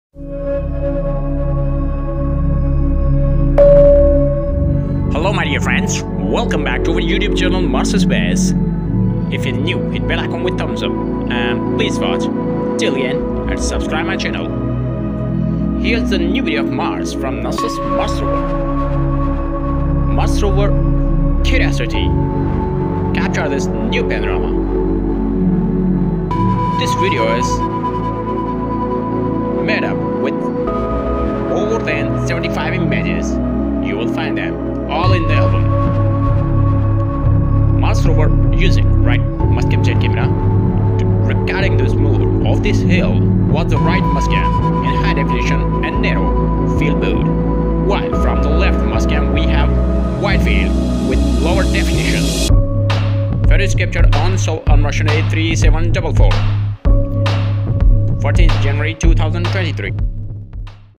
Mars Rover Released Latest 4k Video on Mars || New Mars Video || Mars In 4k: Mars 4k New Video
Mars is the fourth planet from the Sun -a
dusty, cold, desert world with a very thin
atmosphere. Mars is also a dynamic planet
with seasons, polar ice caps, canyons,
extinct volcanoes, and evidence that it was
even more active in the past.
Mars is rich in minerals that formed in water, and
in some cases still hold water. These aptly named
hydrated minerals can be found around the planet,
as shown in maps made using data from NASA's
Mars Reconnaissance Orbiter and the European
Space Agency's Mars Express.
So NASA send many rovers and orbiters to planet mars, and they are searching sign of life on Mars.
and exploring new discoveries of Red Planet.
recently NASA send Mars Perseverance Rover and its still active at the surface of Mars in area of Jezero Crater.
Perseverance has finally collected samples
from the site of an ancient river delta, which
is full of rock layers that serve as a
geological record of the Martian past. Some
of the rocks include the highest
concentration of organic matter found by the
rover to date, according to NASA scientists.

Thanks!
Mars Space

CREDIT: NASA.GOV,
NASA/JPL-Caltech,
NASA/JPL-Caltech/ASU,
NASA/JPL-Caltech/MSSS,